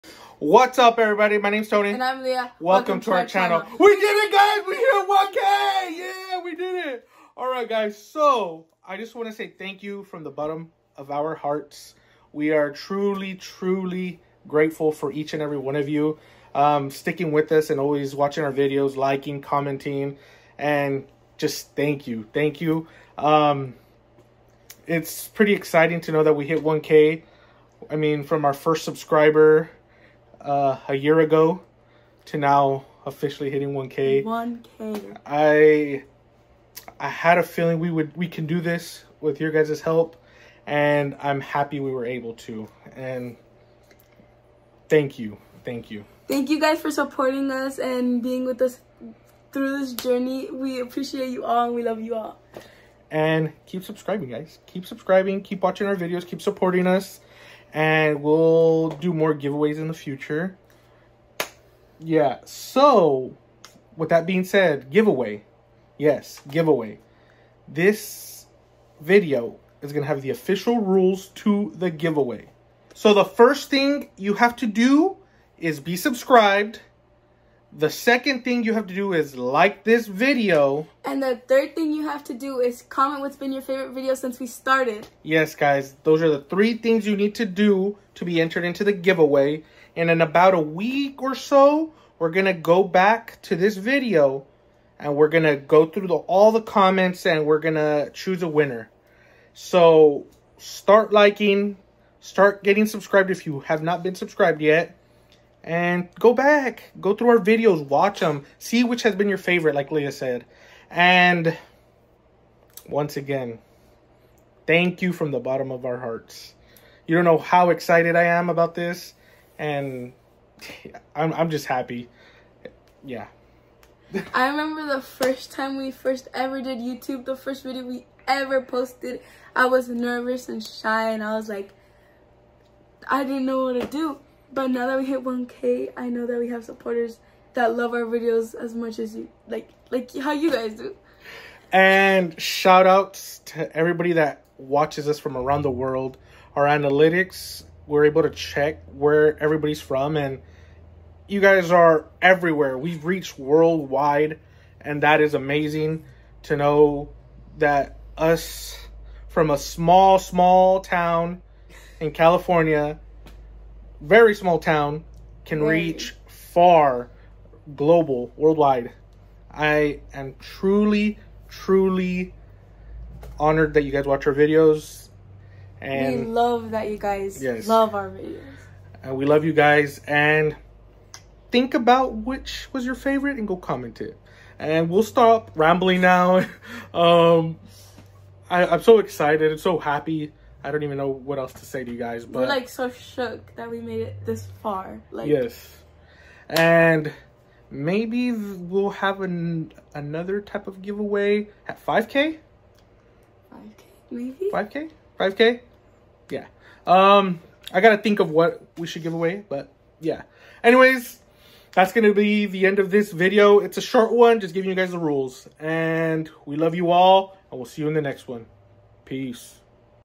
1000 SUBSCRIBER GIVEAWAY 🎉
So todays video is just our rules to our giveaway WE APPRECIATE each and everyone of you thank you for 1K

We Hope You All Liked Todays Video